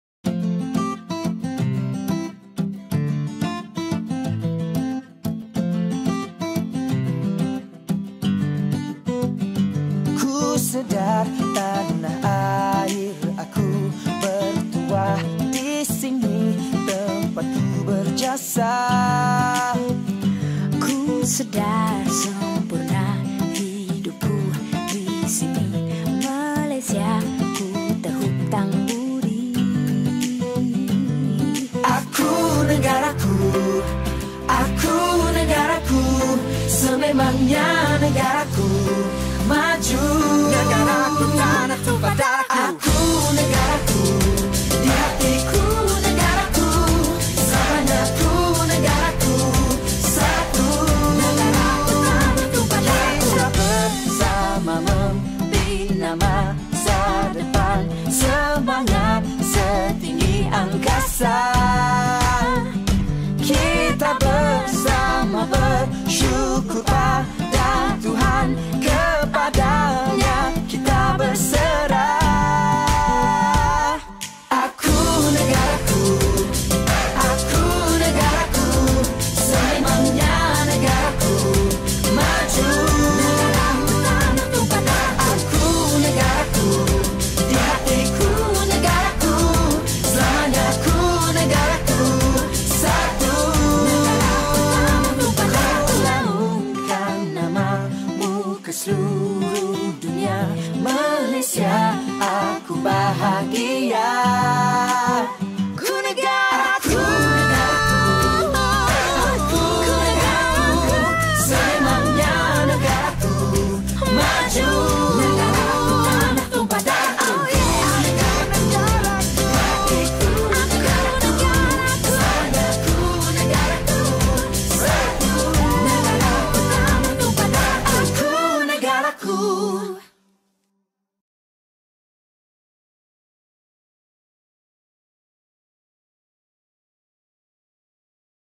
sambutan bulan kemerdekaan sktaam 2017
sambutan kemerdekaan sktaam
sk tun abdul aziz majid
hulu langat
independence day in malaysia primary school